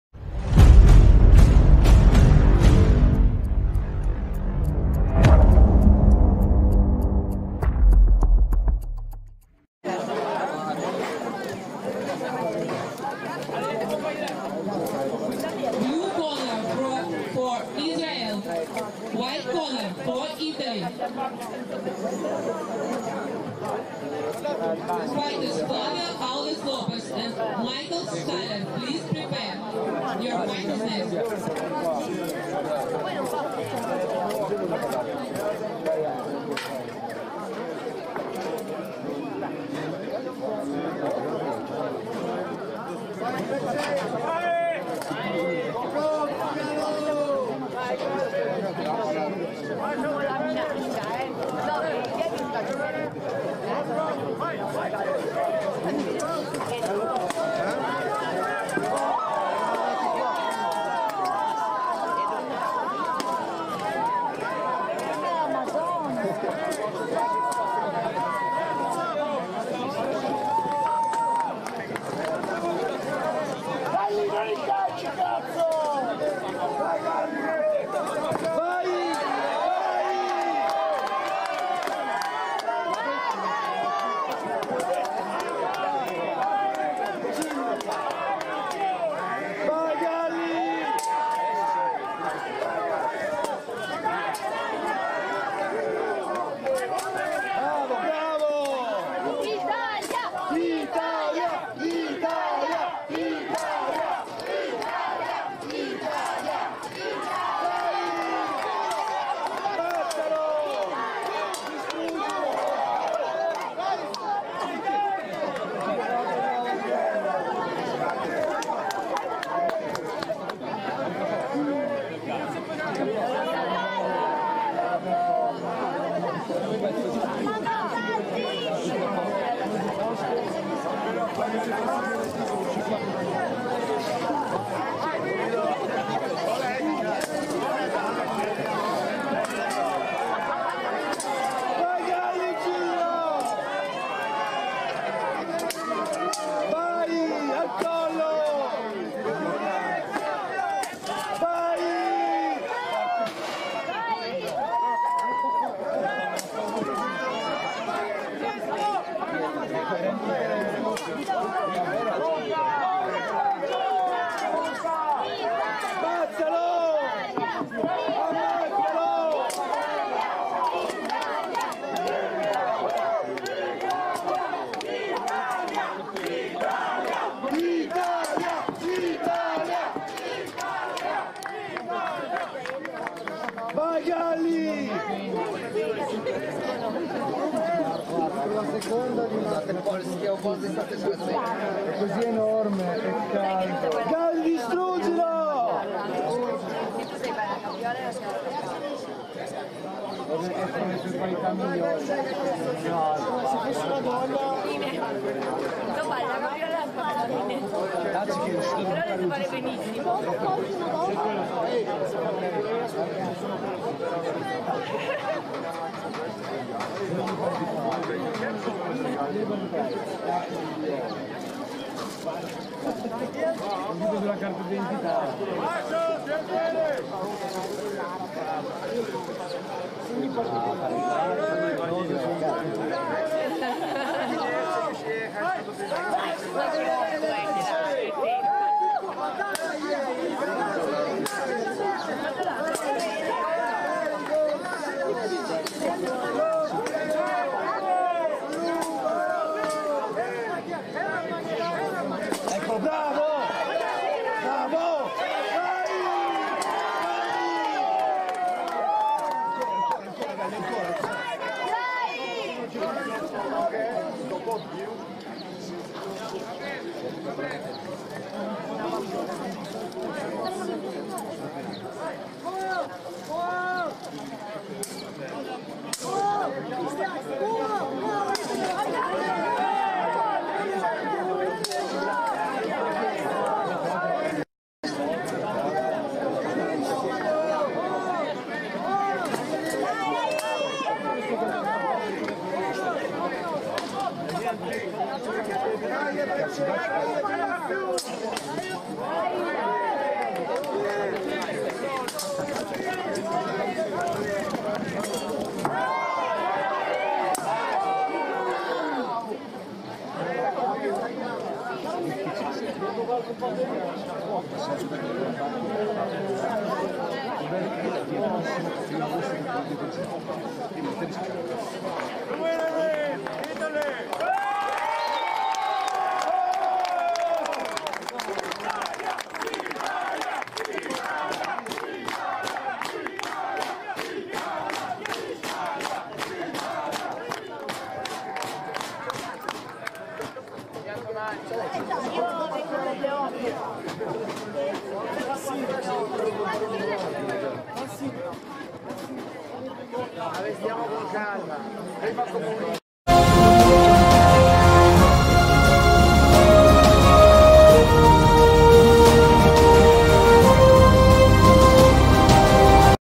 Italy vs Israel 1vs1 Profight Lightweight BOTN 2018
Incontro di Profight che vede l'Italia contro l'Isralele nella categoria Lightweight presso il Castello di Santa Severa al BATTLE OF THE NATIONS 2018.

Profight meeting that sees Italy against Israel in the Lightweight category at the Castle of Santa Severa at the BATTLE OF THE NATIONS 2018.